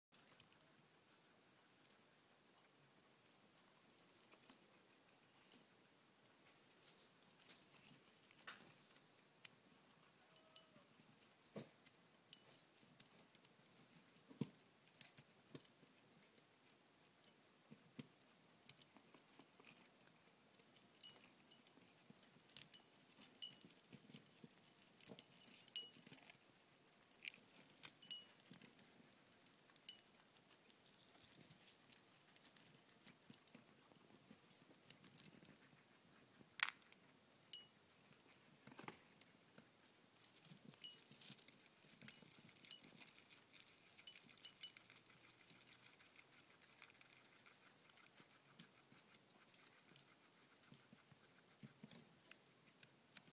Guinea girls salad
My guinea pigs love to eat fresh salad and it's fun to give them a challenge sometimes